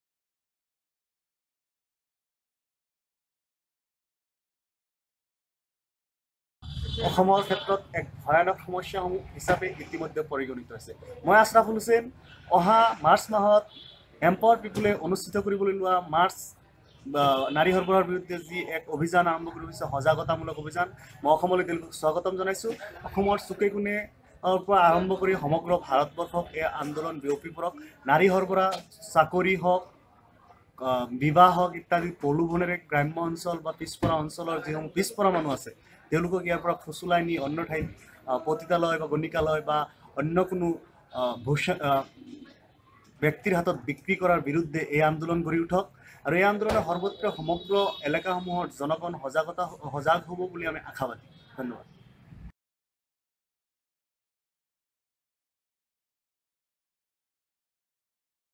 Ashraful message on MABT 2018
there were more than 63 million women “missing” from its population and that 2 million go “missing” across age groups every year because of abortion of female fetuses, disease, neglect and inadequate nutrition. There are also 21 million unwanted girls, Sociologists have long argued that India’s son preference not only occurs in poor rural families but also in middle and upper-middle classes, where tradition dictates a son will carry on the family business or inherit property, though legally, a daughter can do so, too. In the northern farming states of Punjab and Haryana, for example, the sex ratio among infants to 6-year-old is 1,200 men per 1,000 women, even though they are among the wealthiest states.

Such gaps fuel trades like bride trafficking which not only violates human rights a women but also put all of us in dangerous situation. EMPOWER PEOPLE's proposed March is about investigating and understanding cultural phenomenon and to create a mass movement against Bride trafficking and its contributing factors.